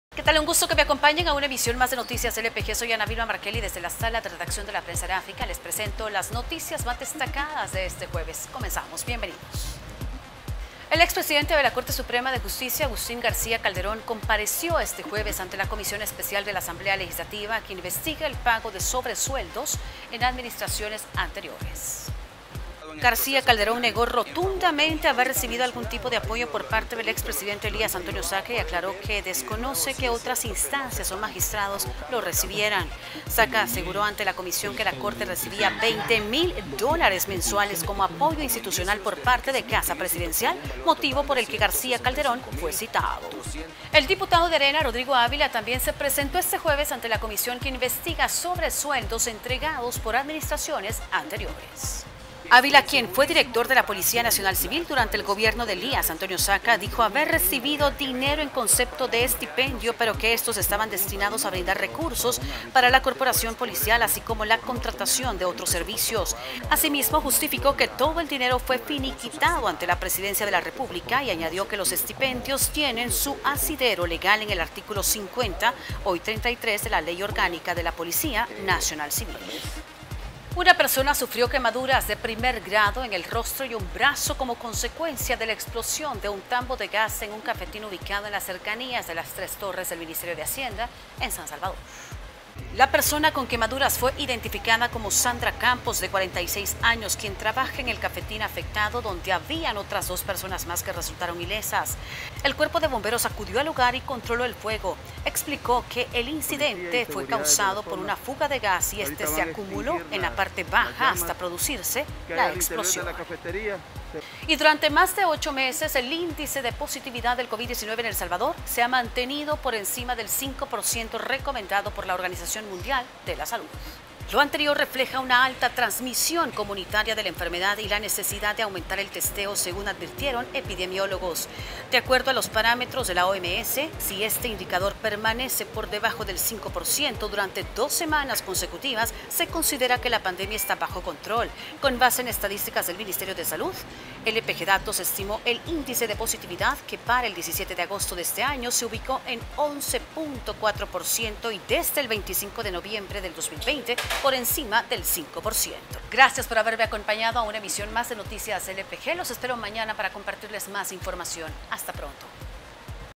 Noticiero LPG 19 de agosto: Rodrigo Ávila asegura ante comisión que recibió fondos con base legal
El diputado por Alianza Republicana Nacionalista (ARENA), Rodrigo Ávila, se presenta este jueves ante la comisión especial de la Asamblea Legislativa que investiga sobresueldos entregados por administraciones anteriores de la partida secreta de Casa Presidencial.

¡Visítenos!